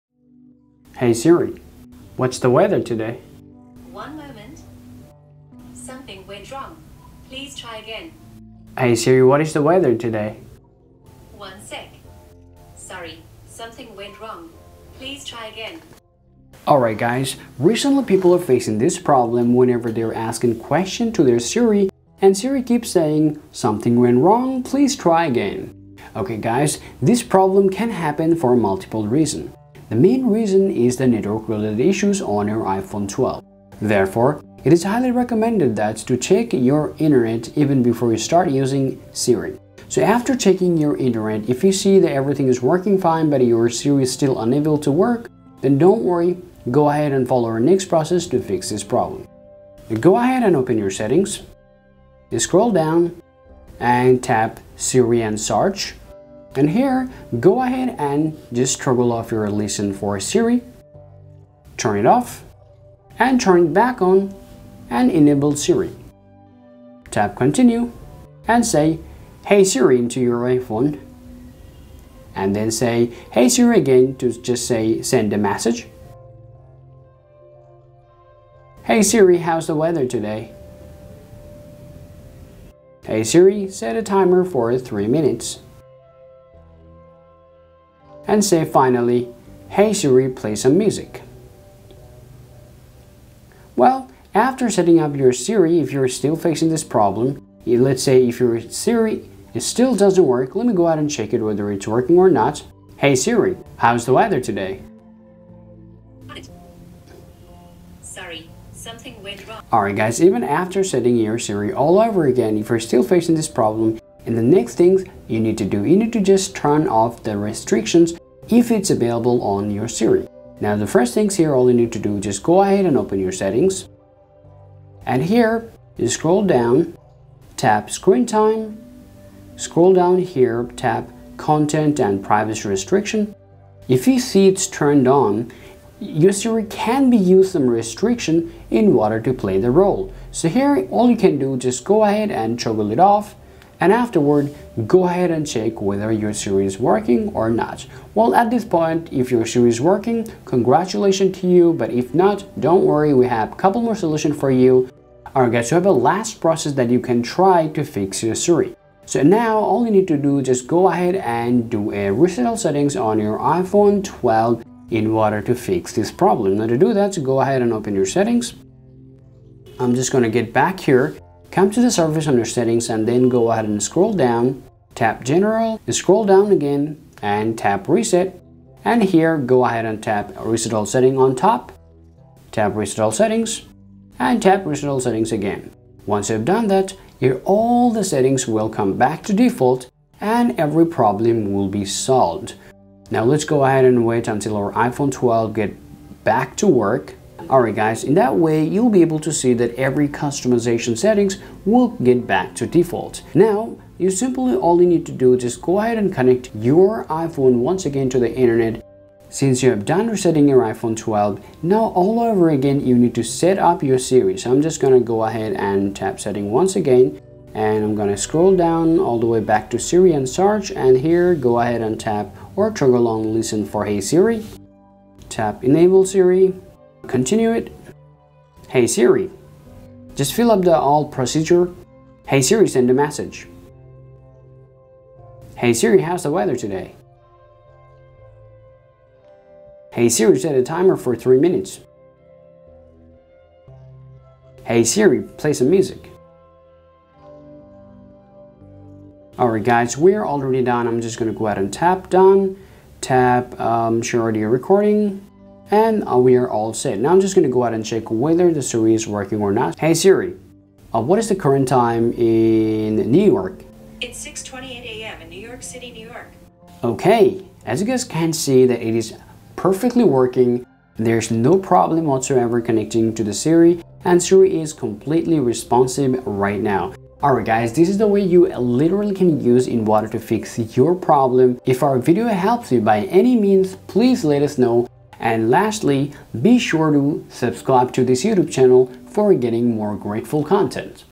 Siri not Working on iOS 14? Here’s the Quick Fix
If your Siri not talking back or responding or not working at all after iOS 14 Update then this video can help you to overcome this situation. With this 4 Process hopefully you can fix SiriNotWorking Problem on iPhone, This Process will work in latest new iPhone 12, 12 Pro, 12 Pro Max.

0:00 Intro
0:35 Process 1
0:50 Reset Siri
1:50 Turn off Restrictions
2:43 Reset All Settings
4:19 Ending